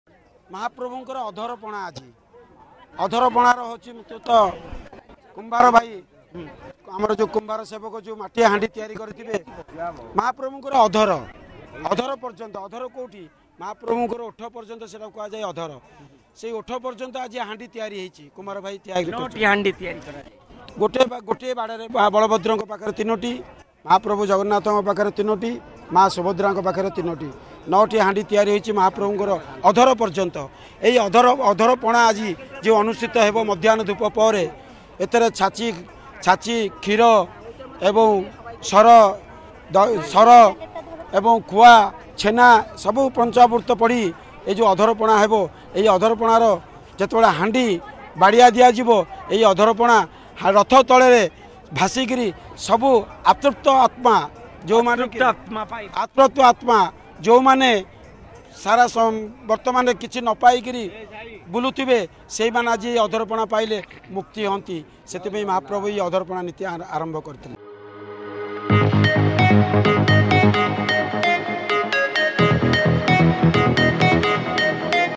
ଅଧରପଣା କଣ ?| Odisha Reporter
ସୁନା ବେଶରେ ମହାବାହୁଙ୍କୁ ଦର୍ଶନ କଲେ ଭକ୍ତ । ଆଜି ଅଧରପଣା ନୀତି ପାଇଁ ଆରମ୍ଭ ହୋଇଛି ପ୍ରସ୍ତୁତି । ତେବେ ଜାଣନ୍ତୁ କାହିଁକି ଲାଗି ହୁଏ ଅଧରପଣା ।